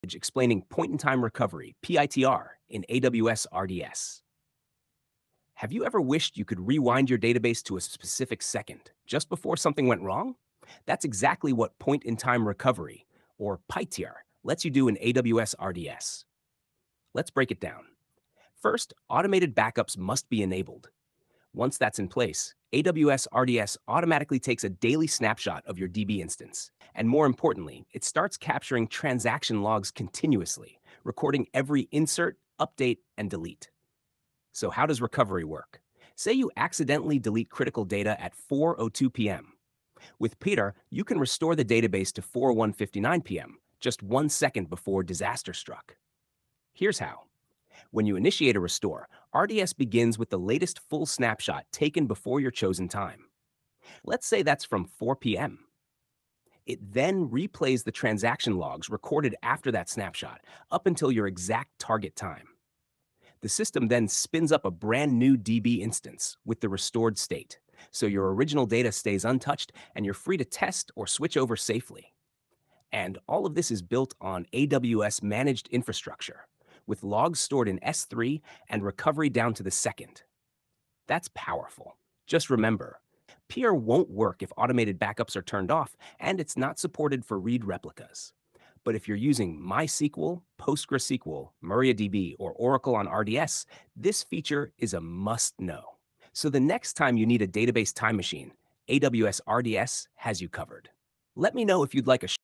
How Point-in-Time Recovery Works in AWS RDS | Full Explanation
With Point-in-Time Recovery (PITR), you can restore your database to the exact second before disaster struck. ⏱️
Learn how AWS uses daily snapshots and continuous transaction logs to make near-instant recovery possible.